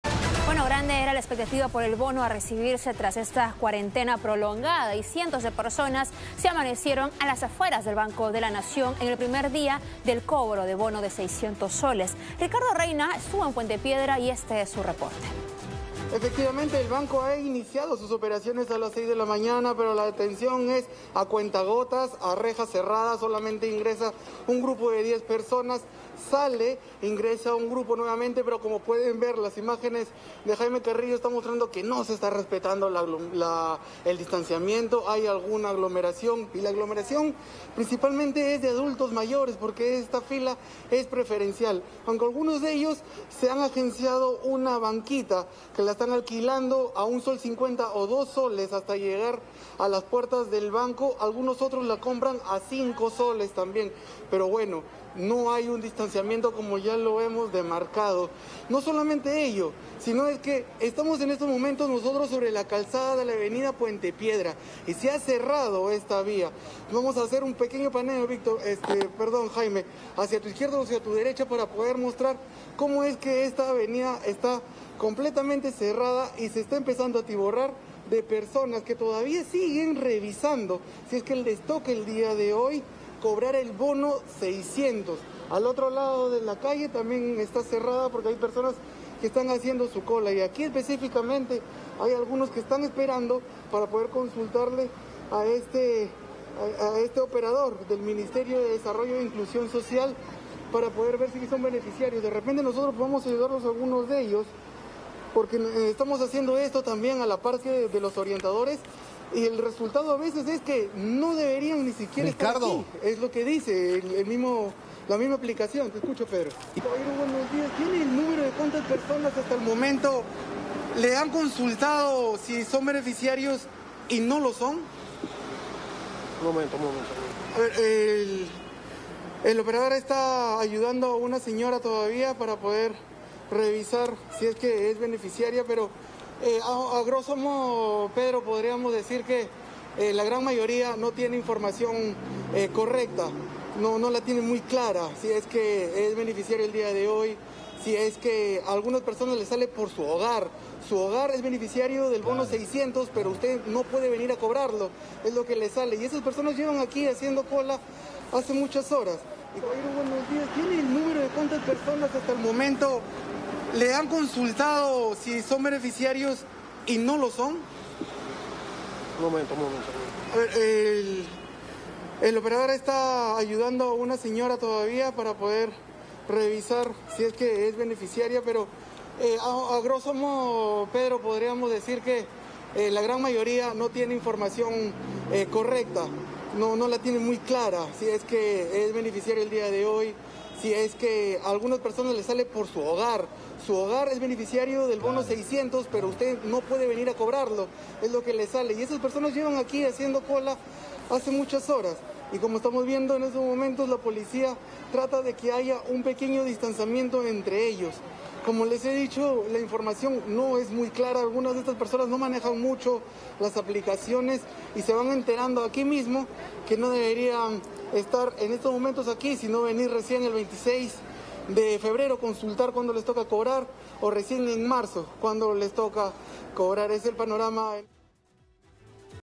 Bono 600 soles: cientos de personas formaron largas colas en Puente Piedra
▶ Varios llegaron a la sede del Banco de la Nación sin saber si eran beneficiarios del bono del Estado.